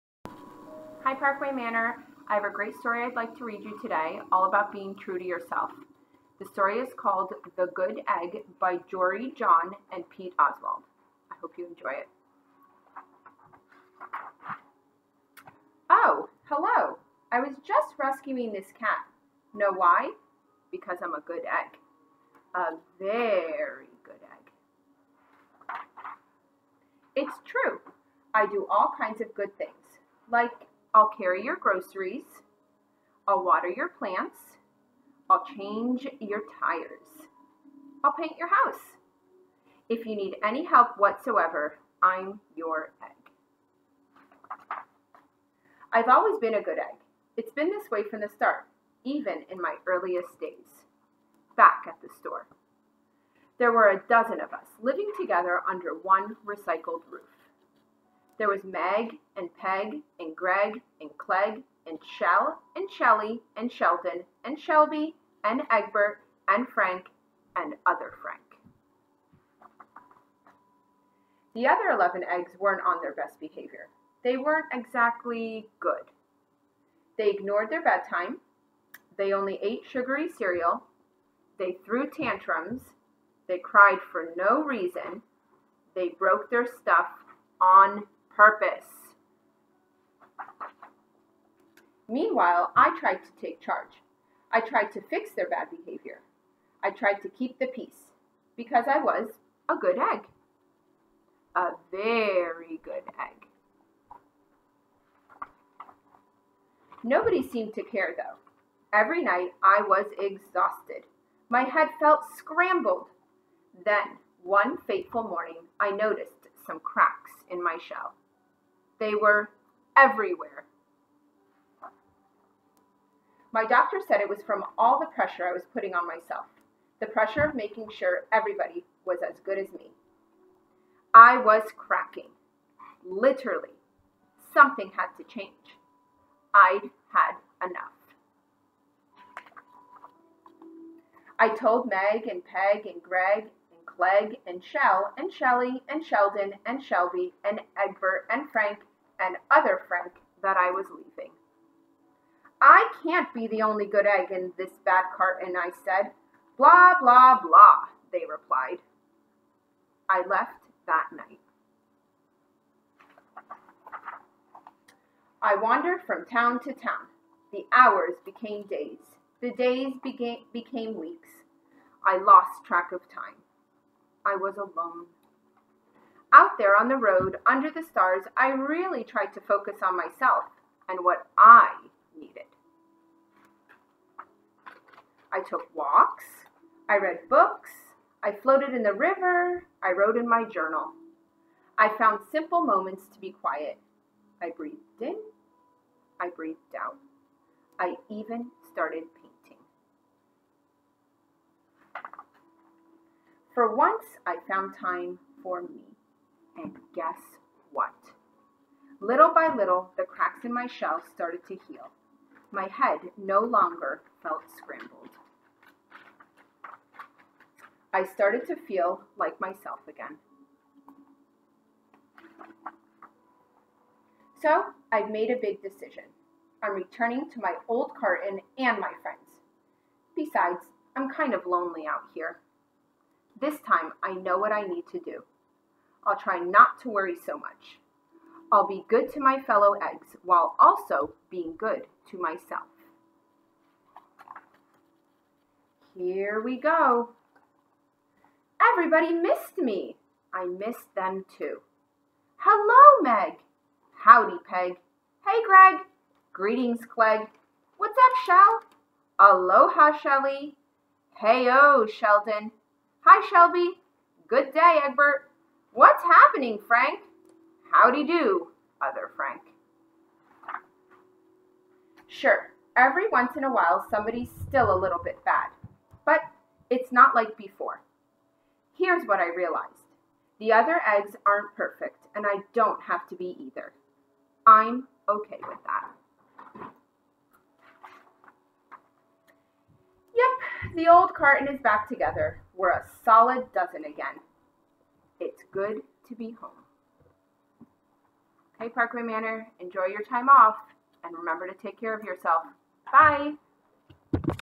Mrs. Cancelliere reads!
Listen to Mrs. Cancelliere read "The Good Egg," by Jory John.